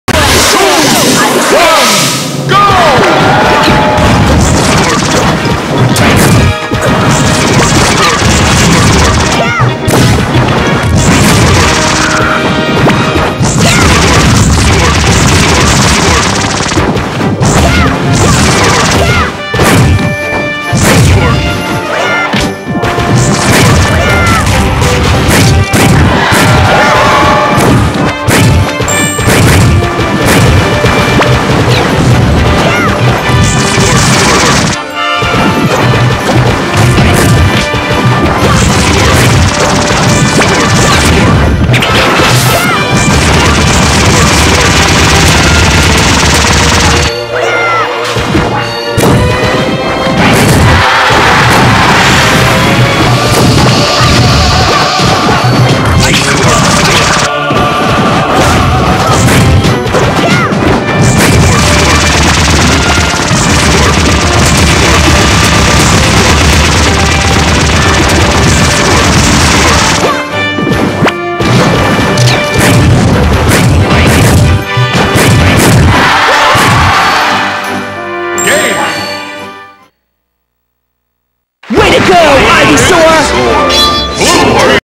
Ivysaur Challenge
Ivy's: Richbrown (@--:) ), Typh (R+T), and Bigfoot (p2)

Bullet seed and upsmash only.

Toon Link = suezo